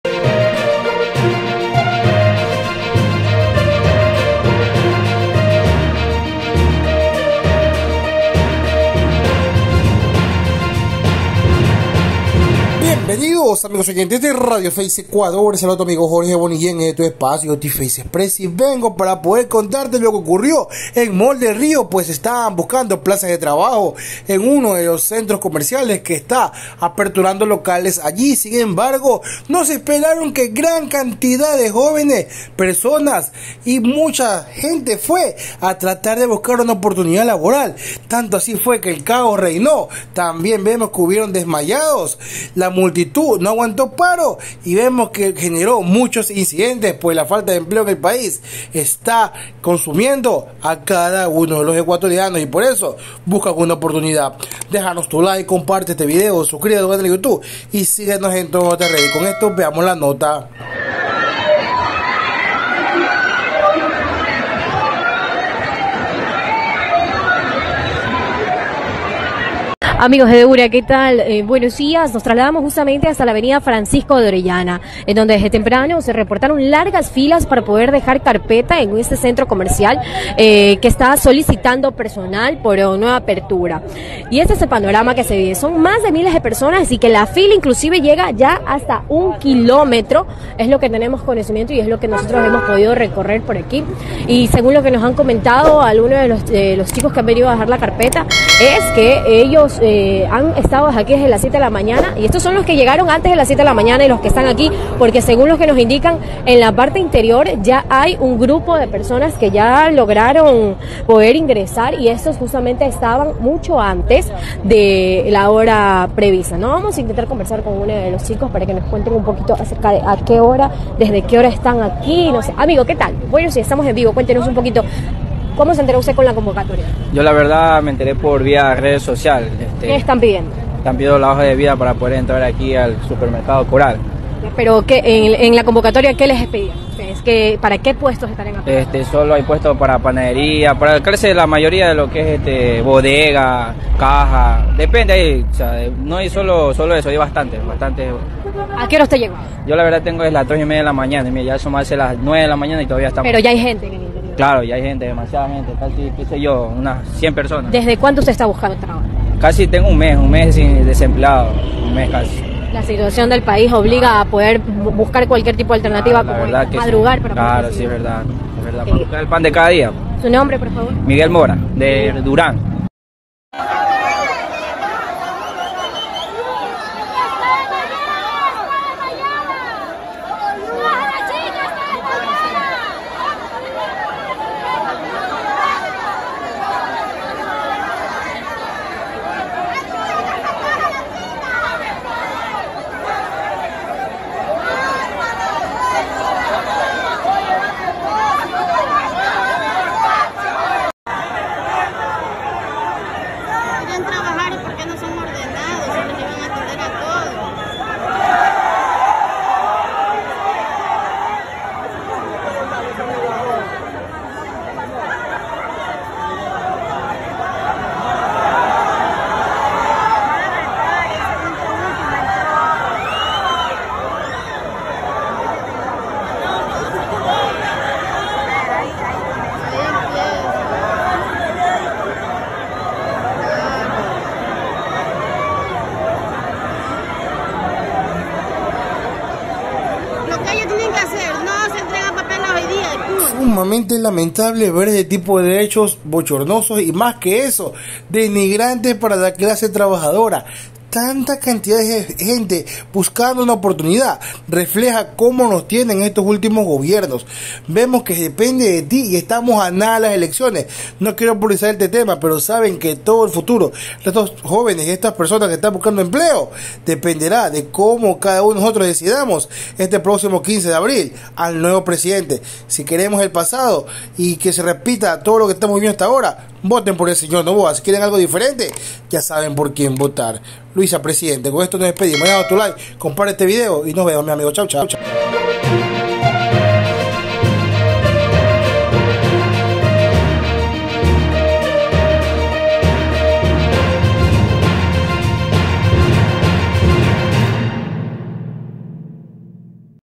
CAOS DESM.ANES Y HASTA DESM.AYADOS EN MALL DEL RIO EN BUSCA DE UN EMPLEO NOTICIAS RFE TV
GRAN CONMOCIÓN EN MALL DEL RIO POR OFERTA DE EMPLEO DÓNDE ACUDIERON MILES DE PERSONAS NOTICIAS RFE TV 👇